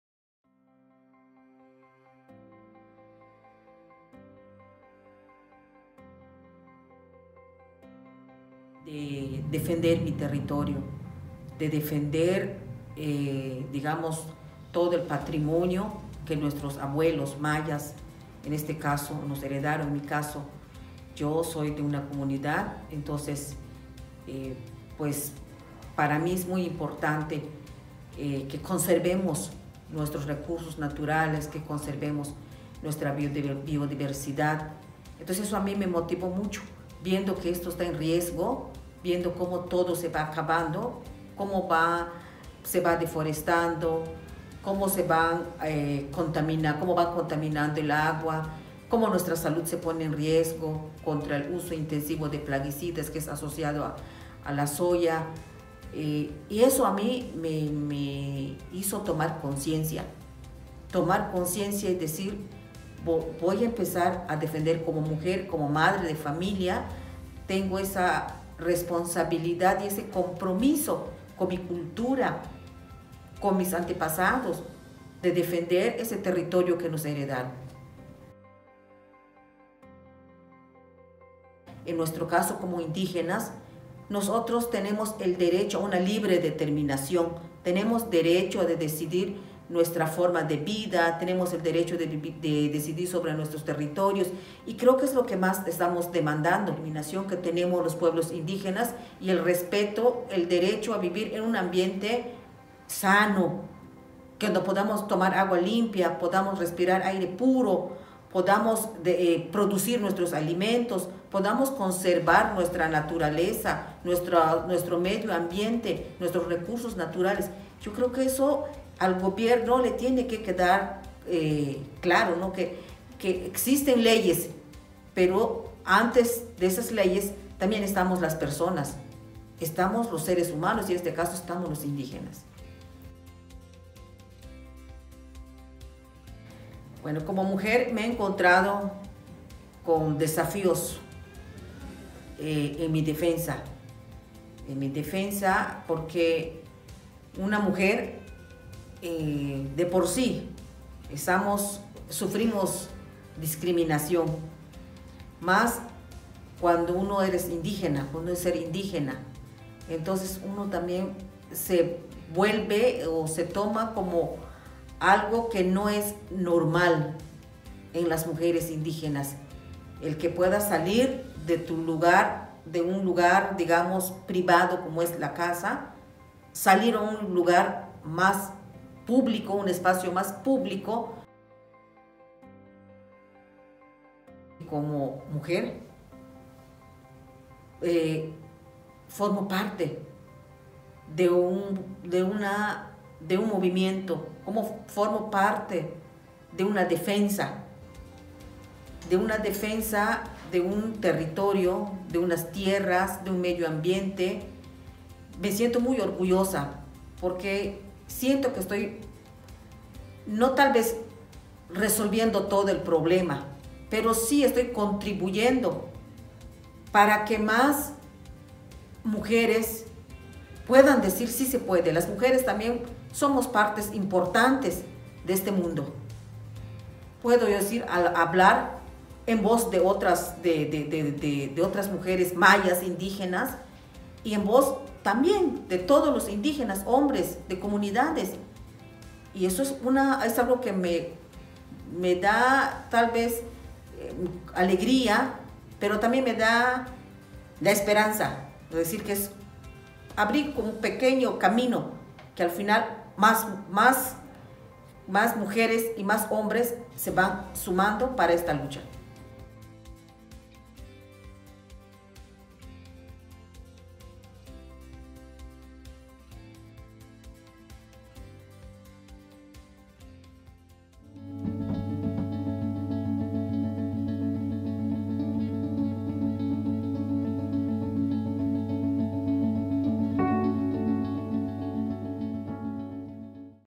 Human rights defender's profile: Leydy Araceli Pech Martin
ISHR was honoured to meet and interview Leydy Araceli Pech Martin from the Colectivo de Comunidades Mayas de Hopelchen.